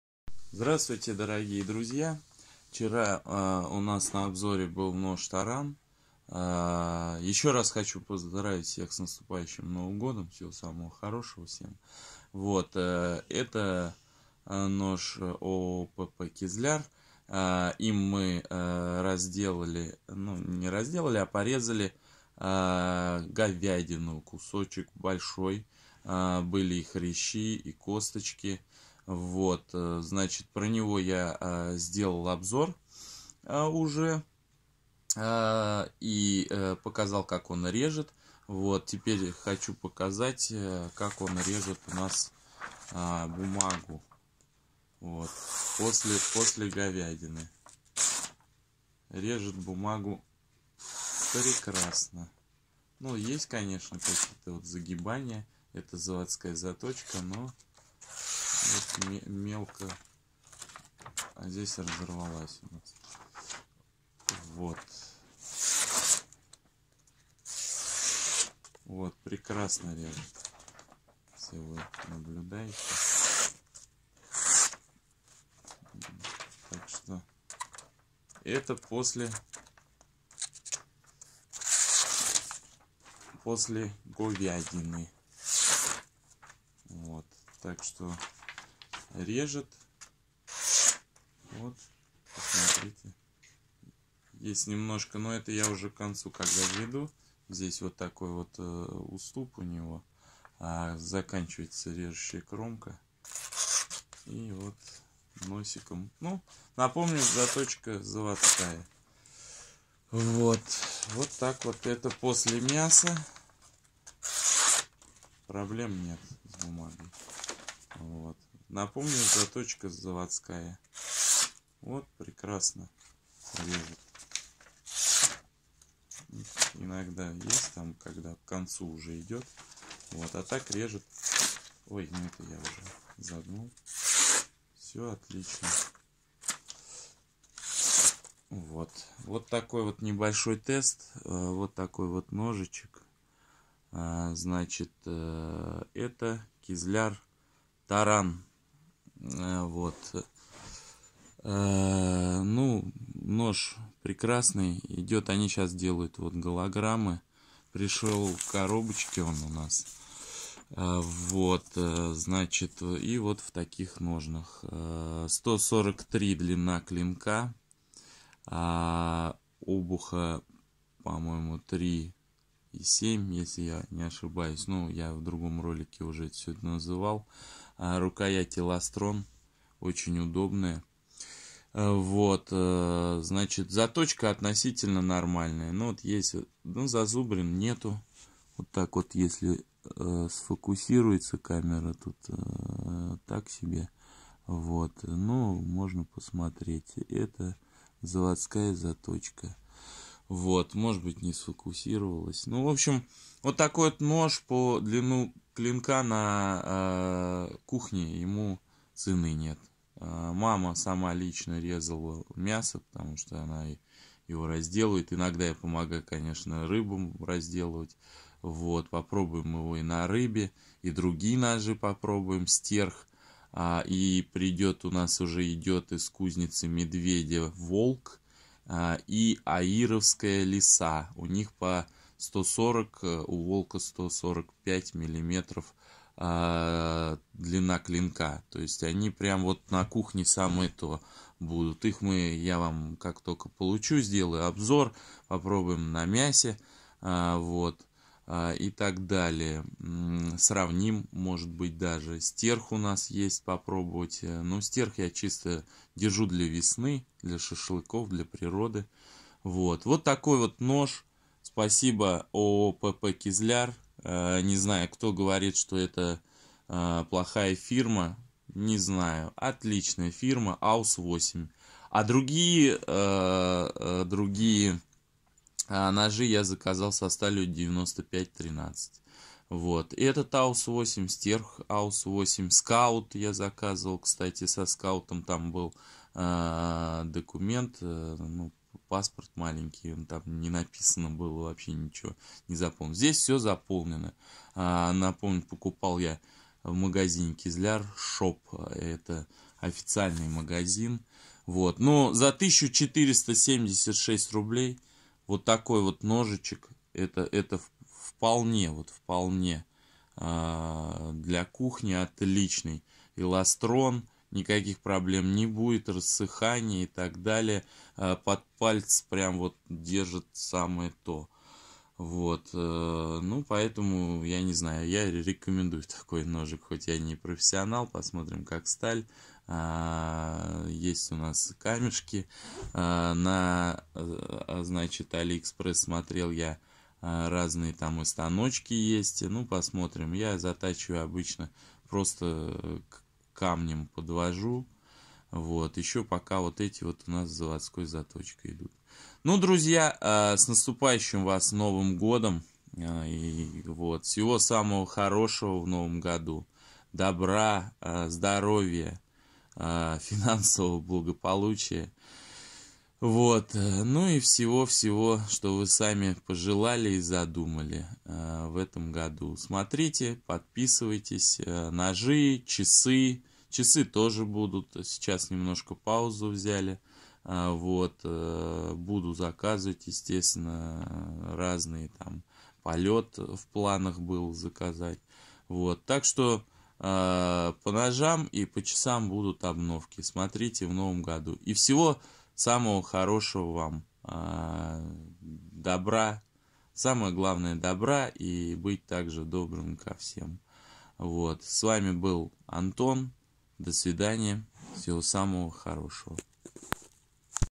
Нож Таран ООО ПП Кизляр
Обзор ножа Таран от Кизляр, рез бумаги после раздели нескольких килограмм мяса!!!
мой адрес в контакте там Вы найдёте большой контент по ножам "Антон Уваров".